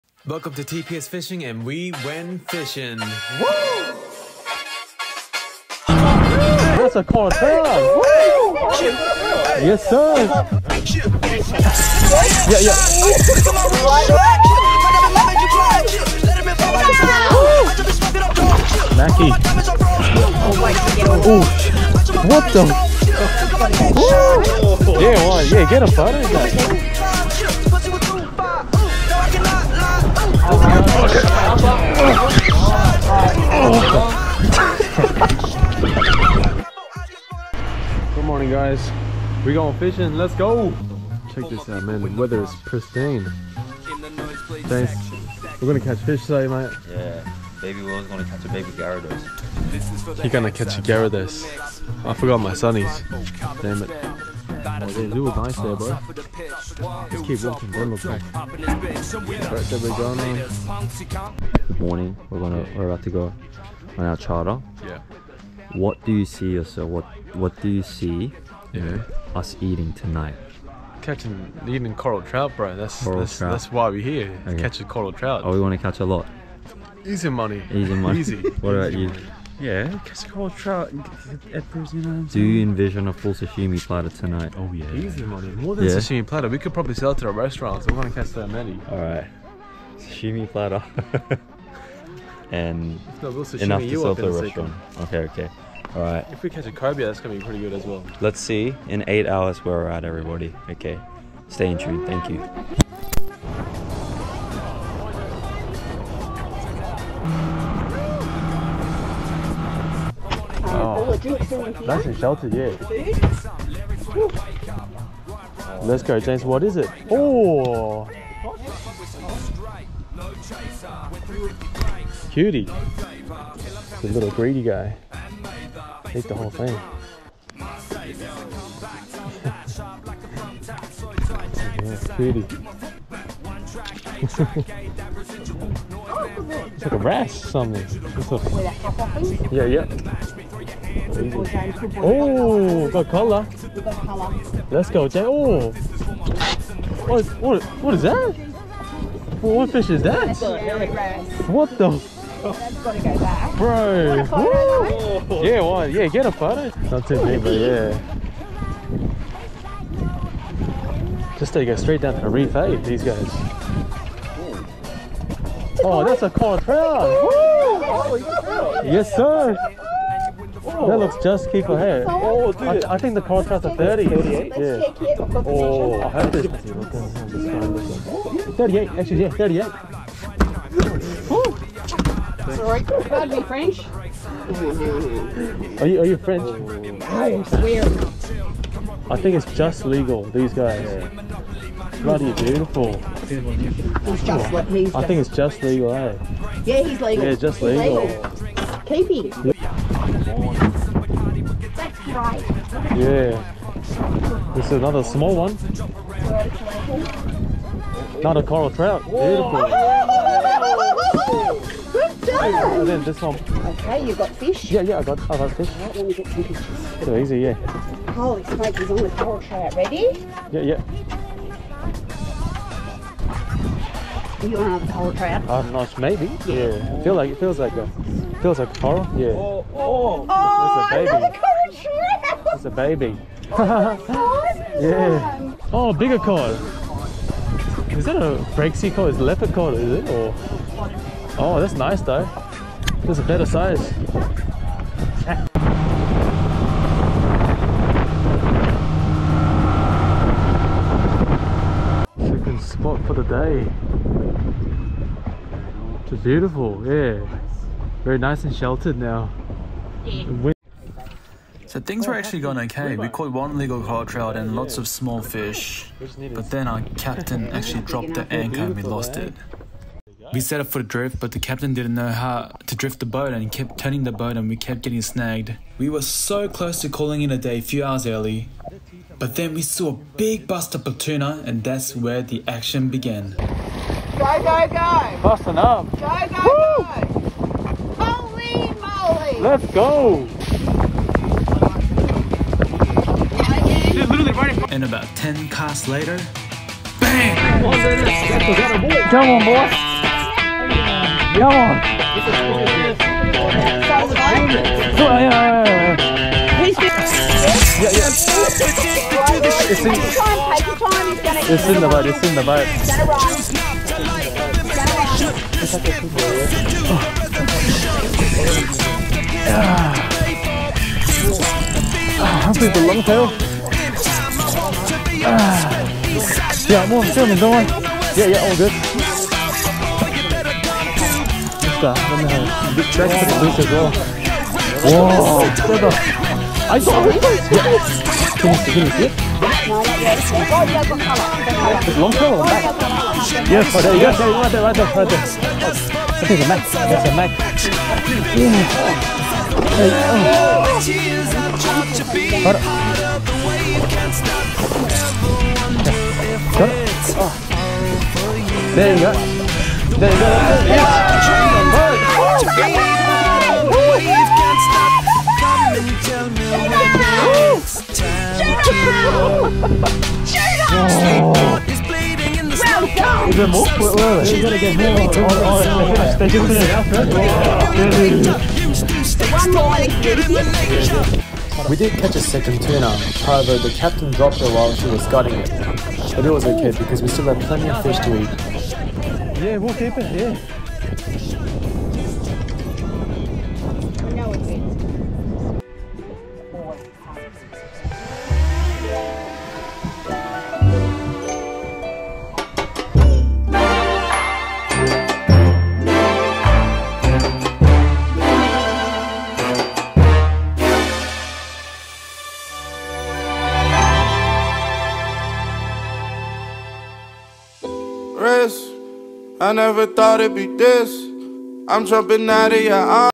I caught my FIRST EVER CORAL TROUT (Whitsundays Fishing Trip EP2)
We went out with Get Out n About Fishing Charters at the Whitsundays. The individual cost for the charter was $385EA inclusive of fishing gear, tackle, food, drinks, gutting, and filleting of the fish you catch. We had a skipper who has never done fishing charter before and this was literally her 3rd time skippering a charter boat. It was a very interesting experience from losing the anchor, moving the boat 180 degrees on a drift, big bust-ups of tuna, and almost calling it a day early.